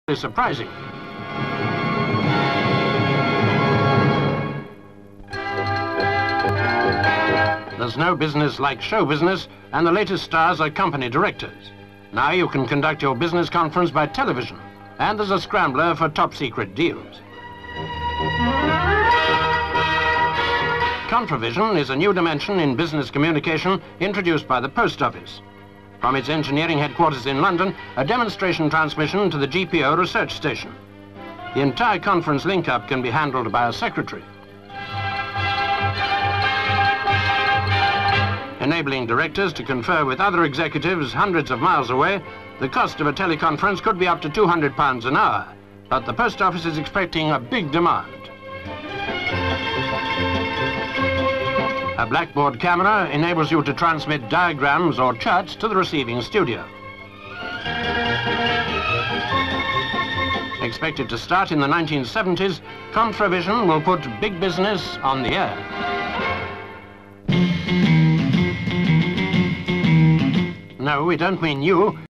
CONFRAVISION
(2 Nov 1967) Preview of the "Tele-Conference", which will link delegates hundreds of miles apart, through sound and vision. A Post Office Service for the 1970's.